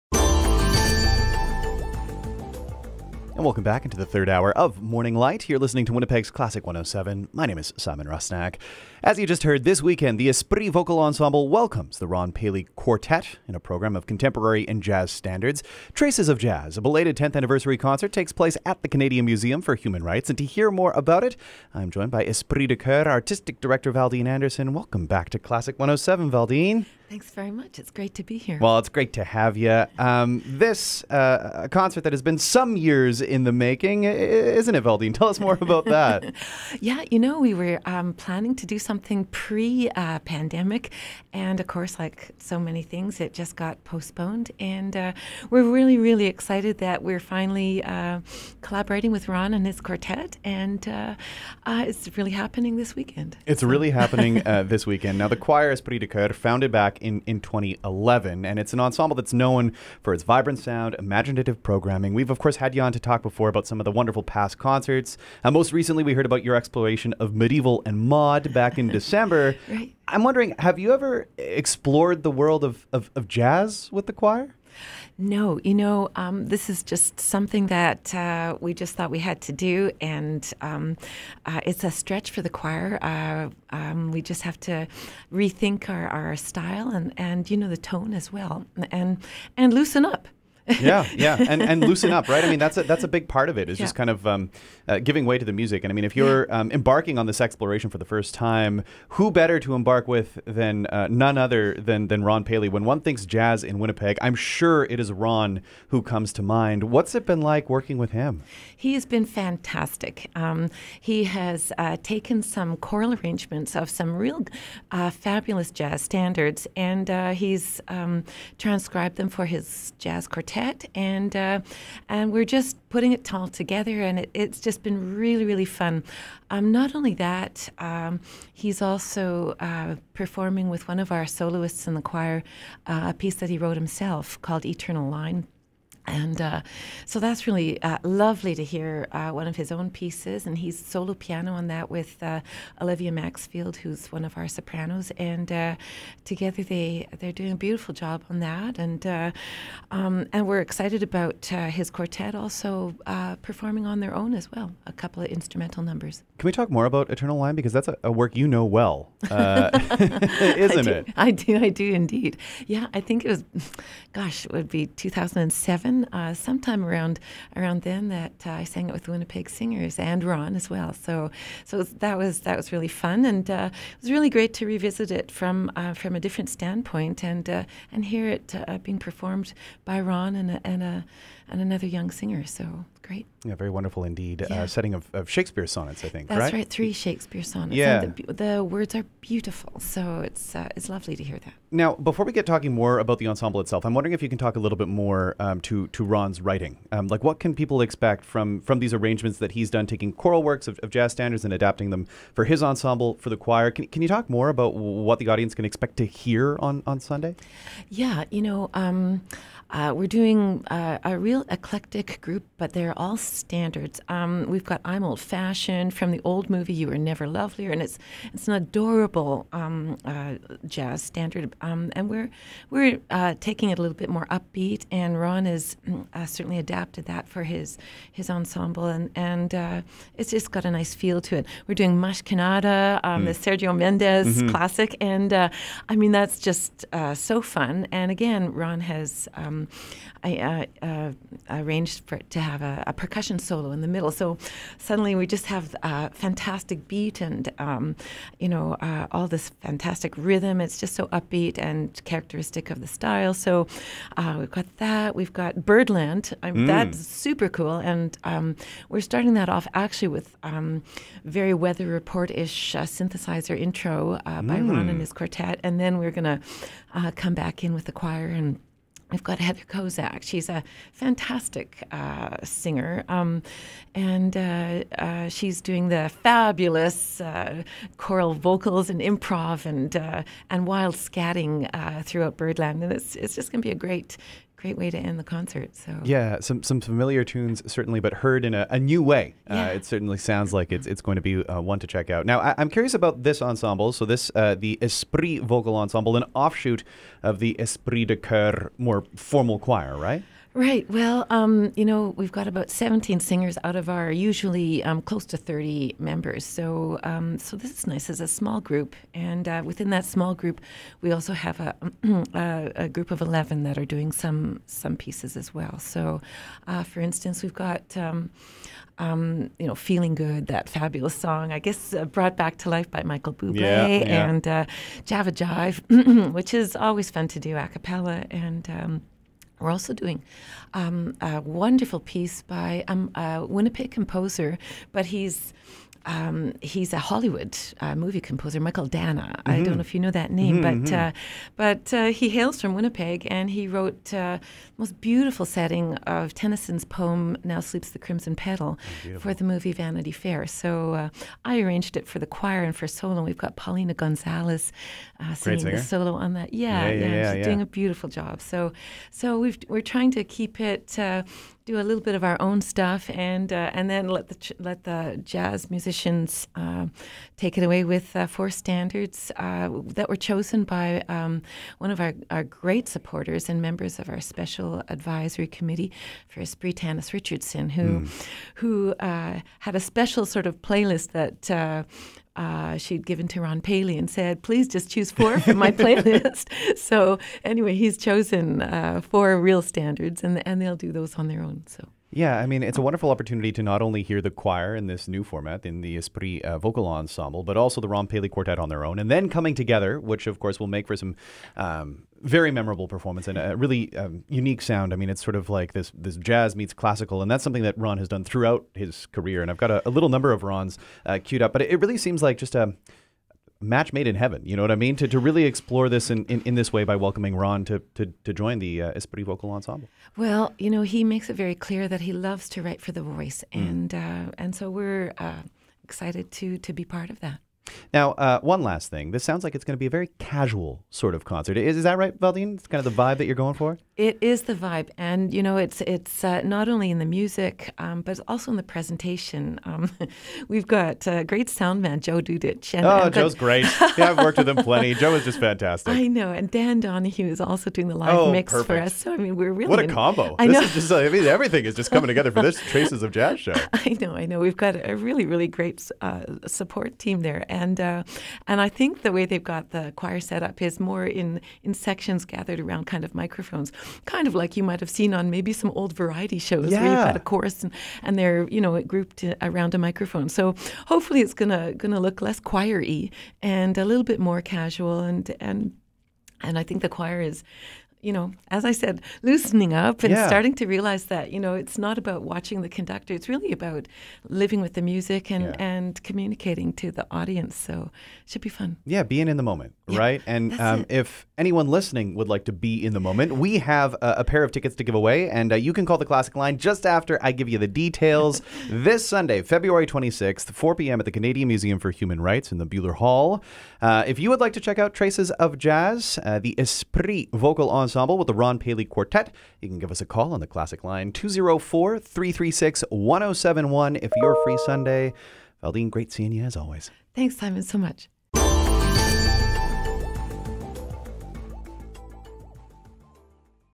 Discover ‘Traces of Jazz’ with the Esprit Vocal Ensemble and the Ron Paley Quartet
This weekend, the Esprit Vocal Ensemble welcomes the Ron Paley Quartet in a program of contemporary and jazz standards.